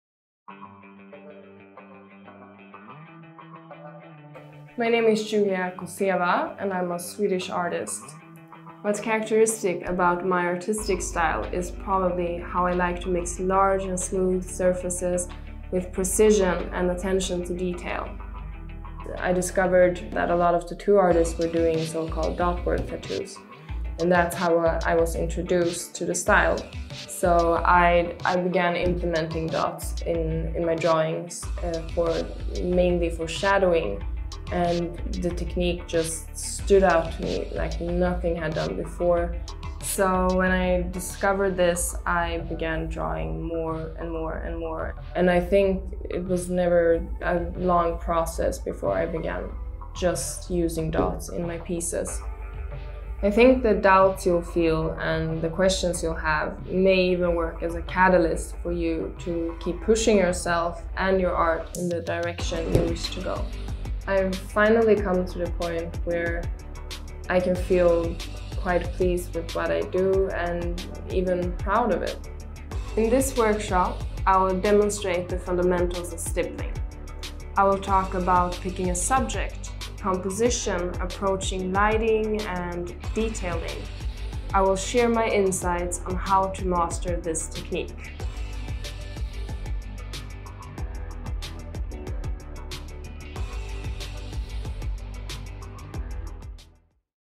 Learn stippling - Julia Koceva
In this video workshop, stippling artist Julia Koceva demonstrates her technique and teaches you all about the stippling process.